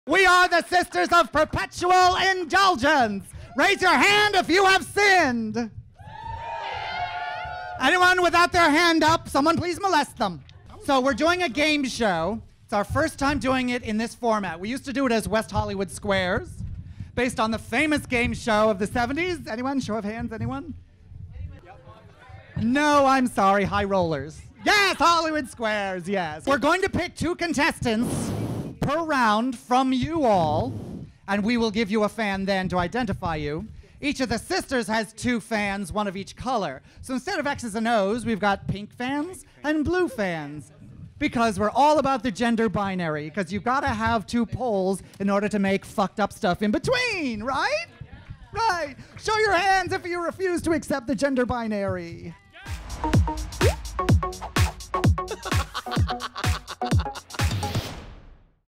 DRAGCON TEASER!: Sisters of Perpetual Indulgence w/ Sister Roma, Sister Unity, Sister Bearoncé Knows
The Sisters of Perpetual Indulgence host a gagworthy game during their panel at DragCon 2016 hosted by Sister Unity with special panel guests Sister Roma, Bearoncé Knows, Sister Faegala Tina Pfischzoot, Sister Deitrich Anne DiMaggio, Sister Twisted Fister of the Holy Crisco, Sister Nora Torious.

The Sisters of Perpetual Indulgence is a charity, protest, and street performance organization that uses drag and religious imagery to call attention to sexual intolerance and satirizes issues of gender and morality.